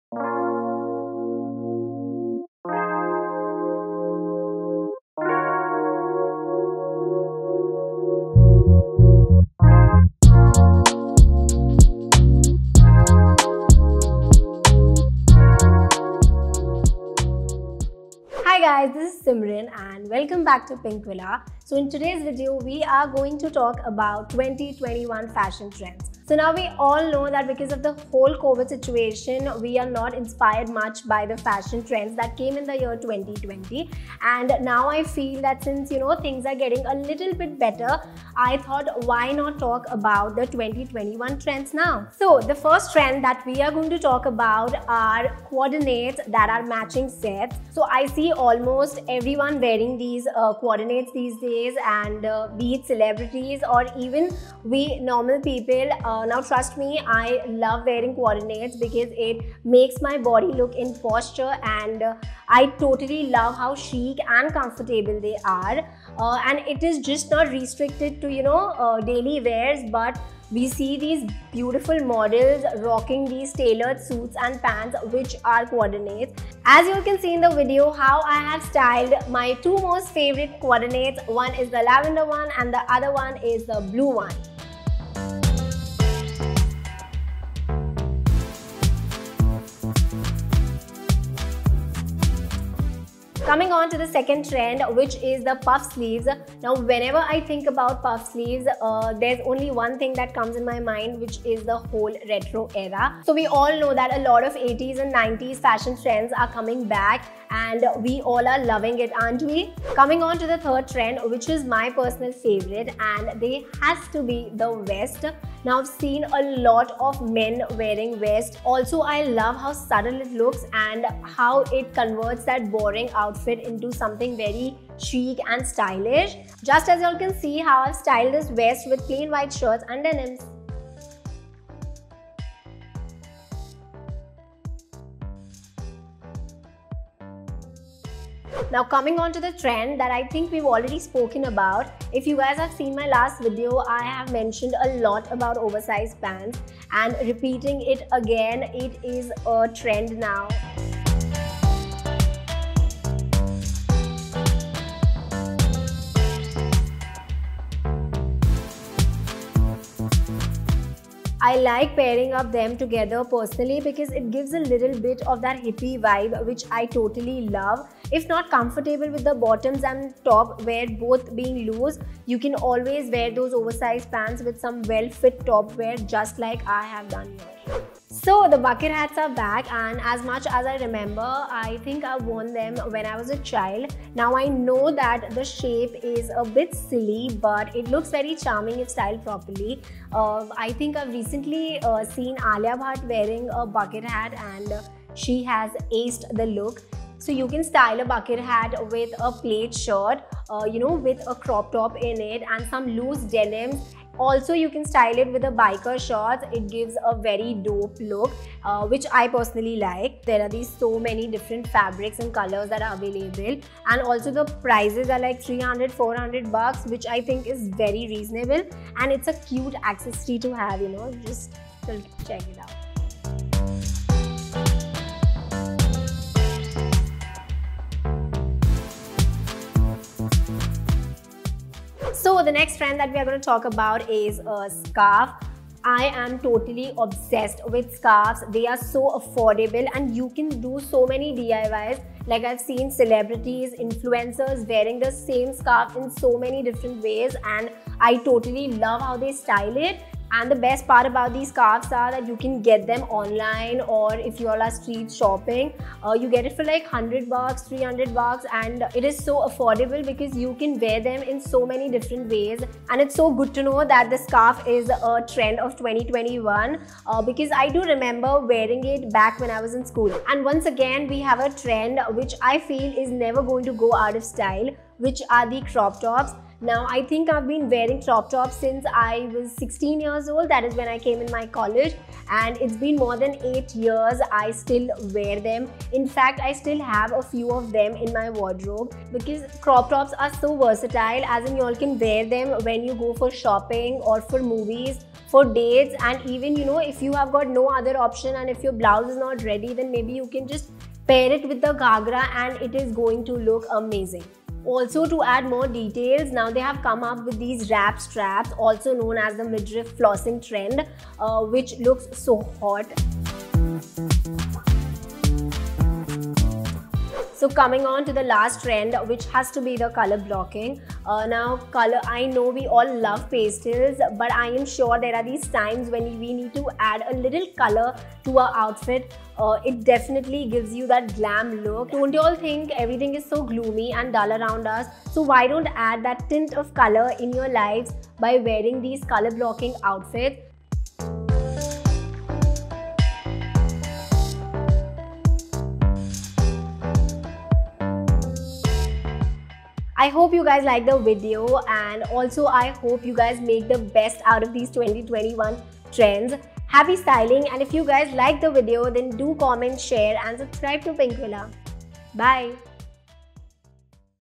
Top 10 Fashion Trends of 2021 | Pinkvilla
Our fashion influencer  Simren Sanghvi tells us about the trending outfits and must haves in 2021